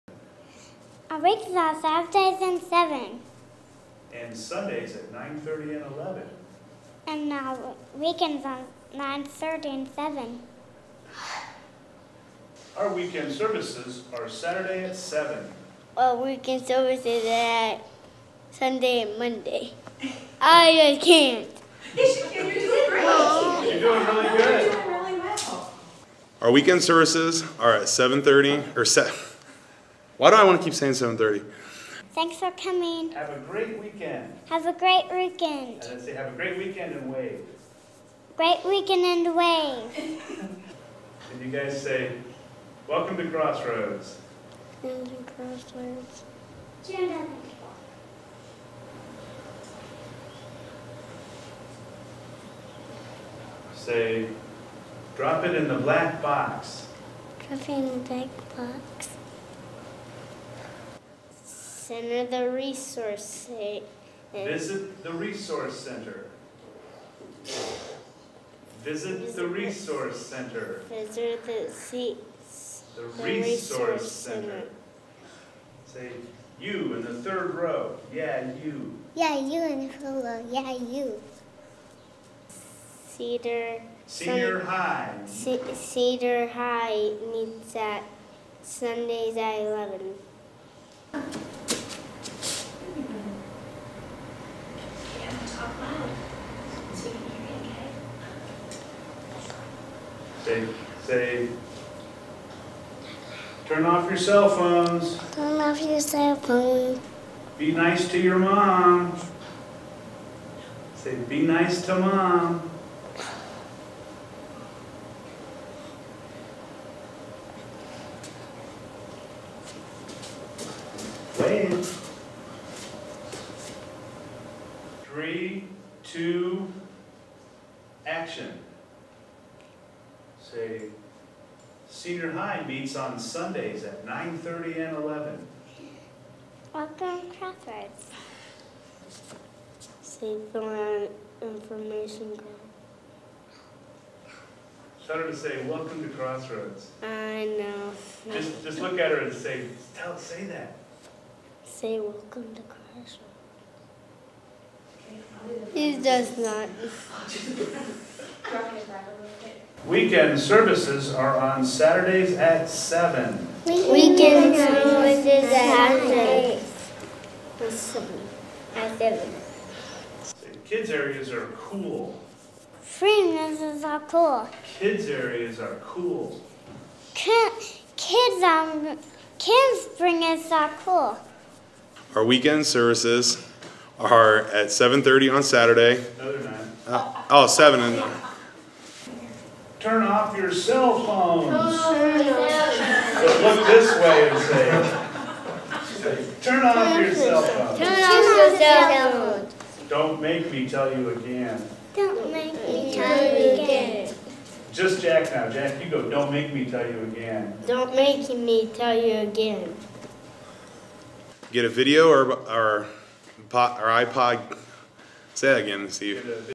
Mother's Day Bloopers
Kids were the feature for our announcements on Mother's Day.  Here's a few that didn't make the final cut...